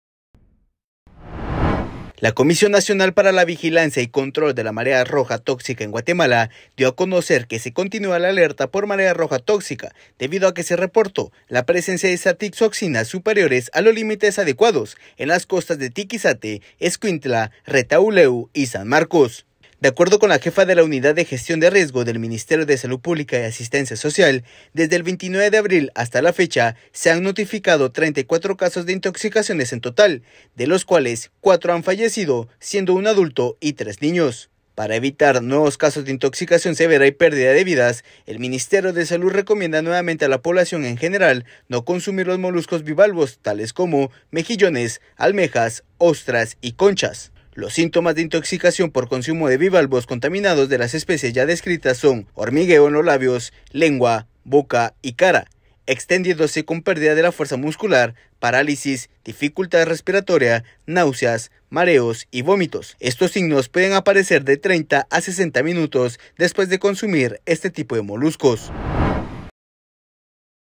Cuatro fallecidos por consumo de moluscos
Hasta la fecha el Ministerio de Salud reporta el fallecimiento de cuatro personas, dentro de ellas tres menores de edad por intoxicación por consumo de moluscos.